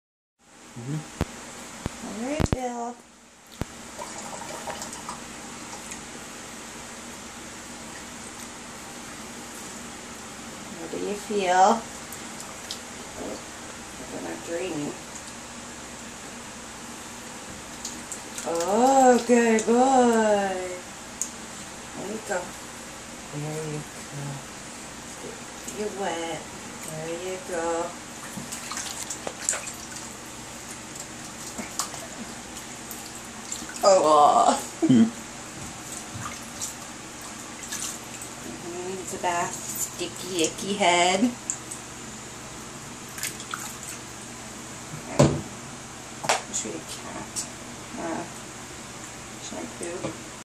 Kitten First bath
Kittens first bath in the sink.